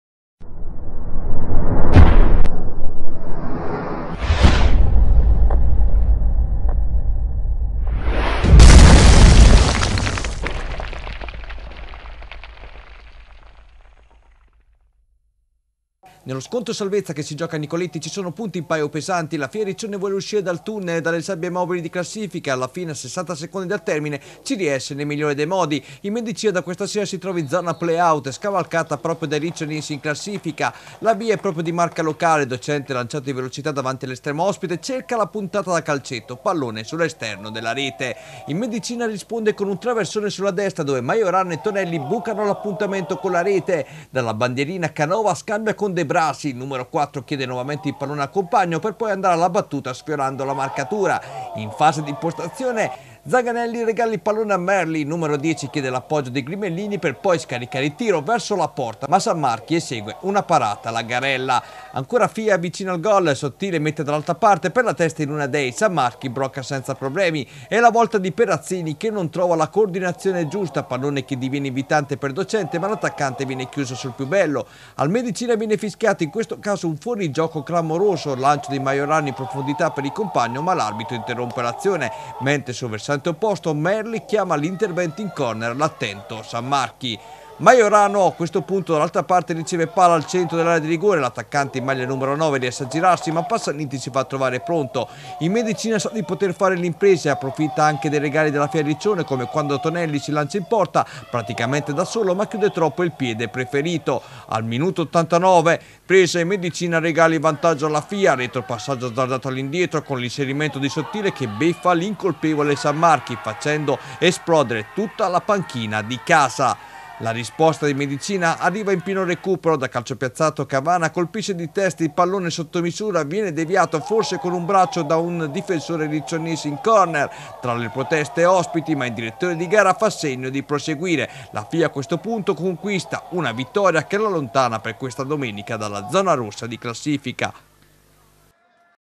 Calcio Eccellenza Romagna Girone B. Fya Riccione vs Medicina 1-0
Servizio e commento a cura di Daniele Manuelli. Riprese coperte da copyright. Riprese a cura di Gianluca Melappioni